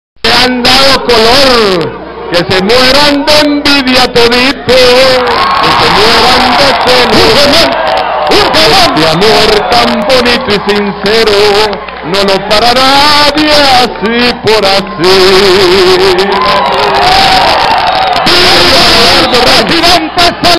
la cuarta urna "va"... dice Mel
en un mensaje que dio el presidente mel zelaya dice que la cuarta va duela a quien le duela.. solo esperamos que las autoridades correspondientes le pongan un alto a este señor y lo UBIQUEN BIEN para que se le bajen los sumos a el  y sus seguidores... DERROQUEN EL GOBIERNO DE MEL...